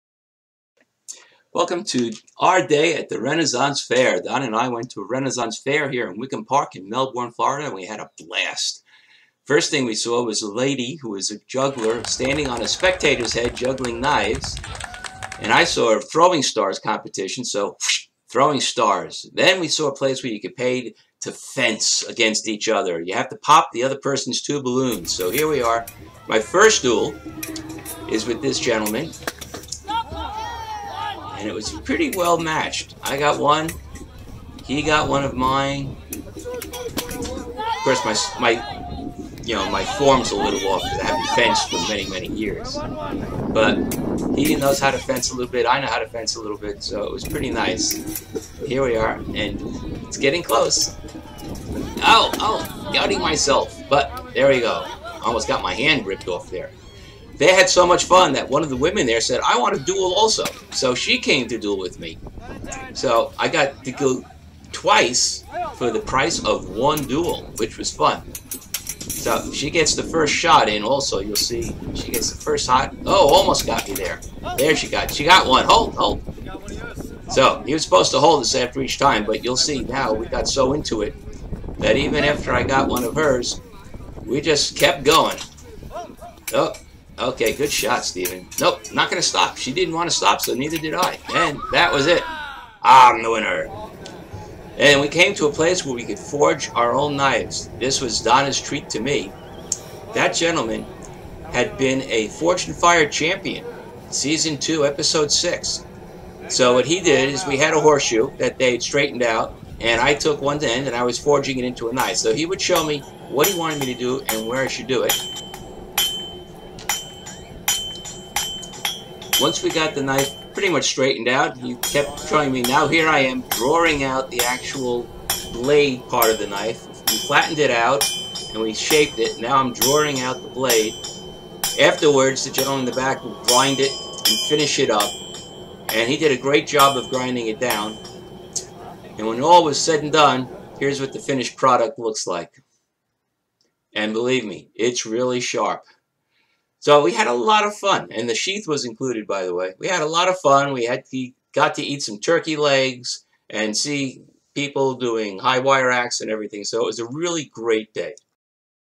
Day at the Renaissance Fair
The Renaissance Fair was at Wickham Park and Donna and I spent the morning there/